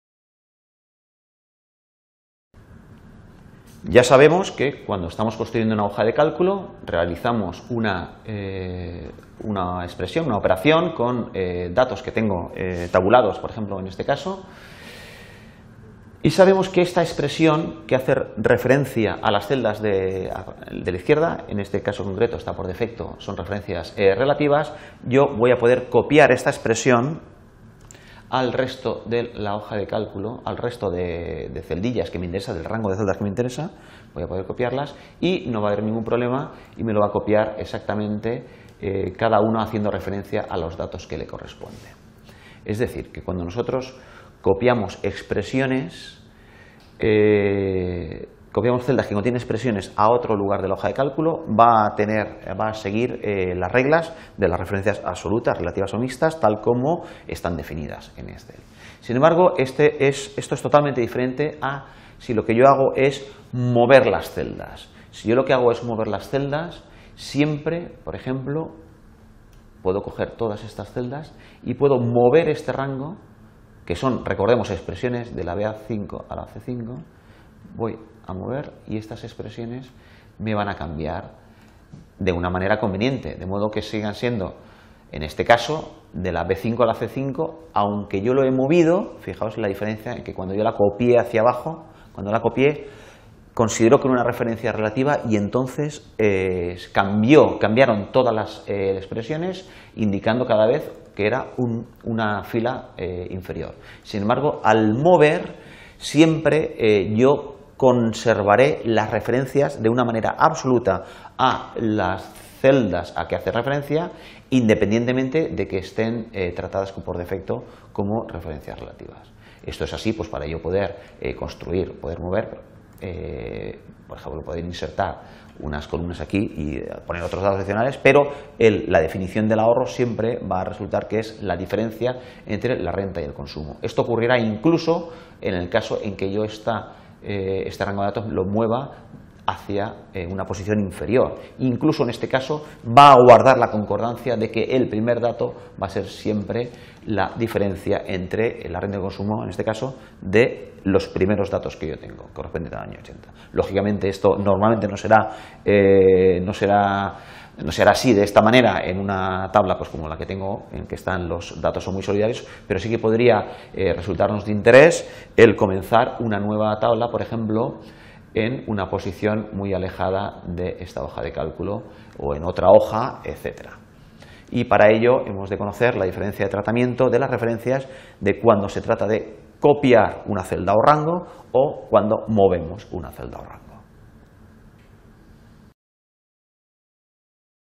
Mover y Copiar celdas o rangos con referencias| UPV
Título: Mover y Copiar celdas o rangos con referencias

Descripción automática: En este video, se explica cómo funcionan las referencias en las hojas de cálculo como Excel, en particular la diferencia entre copiar y mover celdas con fórmulas. Al copiar fórmulas, las referencias a las celdas suelen ajustarse automáticamente en función del nuevo lugar donde se pegan las celdas; estas son referencias relativas. Por el contrario, al mover celdas con fórmulas, las fórmulas mantendrán las referencias originales, comportándose como referencias absolutas, independientemente de que en su posición original fueran relativas.

El vídeo también menciona cómo este comportamiento permite construir y modificar hojas de cálculo de manera flexible, por ejemplo, al insertar columnas con datos opcionales o al empezar tablas nuevas en diferentes ubicaciones de la hoja de cálculo o en otras hojas. Se enfatiza la importancia de conocer la distinción entre copiar y mover para trabajar eficientemente con los datos y mantener la integridad de las fórmulas originales frente a cambios en la ubicación de celdas y rangos.